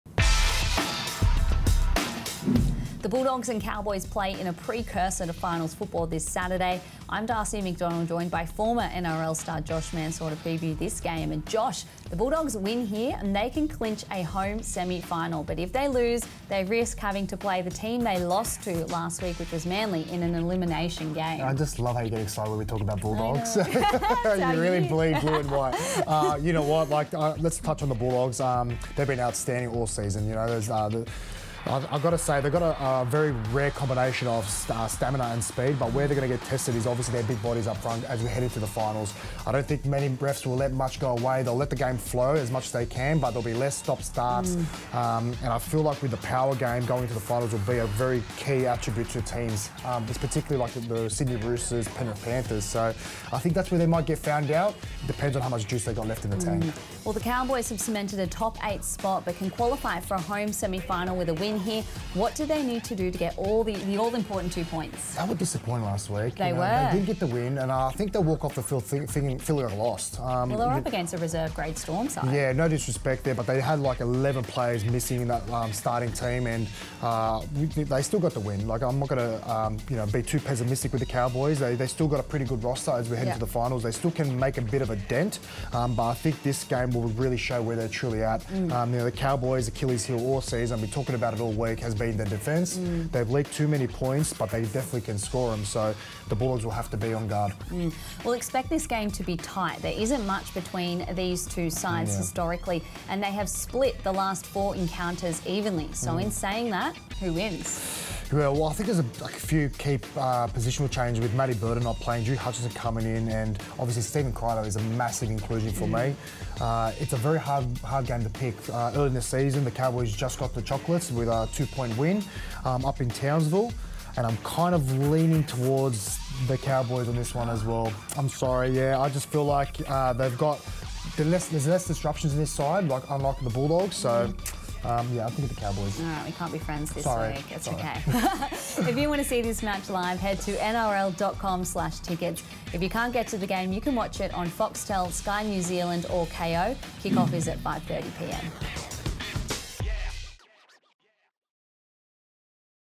NRL 2024 | Bulldogs v Cowboys: Round 27 | Match Preview
Josh Mansour and Darcie McDonald preview the Round 27 match between the Canterbury-Bankstown Bulldogs and North Queensland Cowboys